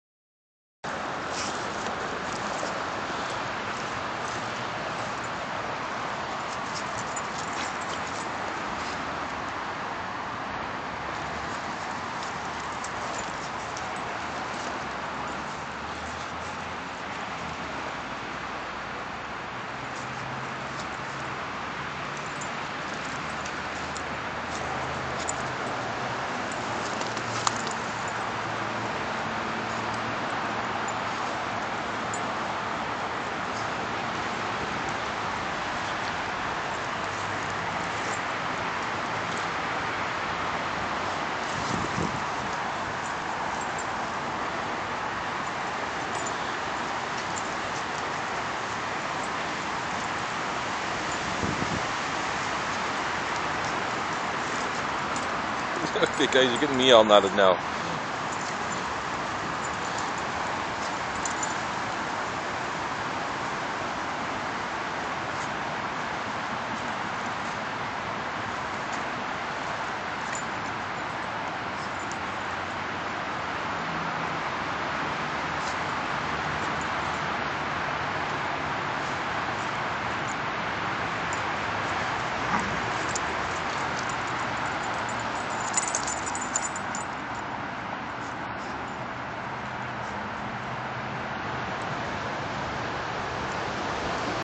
Dog-nip
Stewie and Calvin found an intoxicating area of grass and wouldn't stop rolling in it.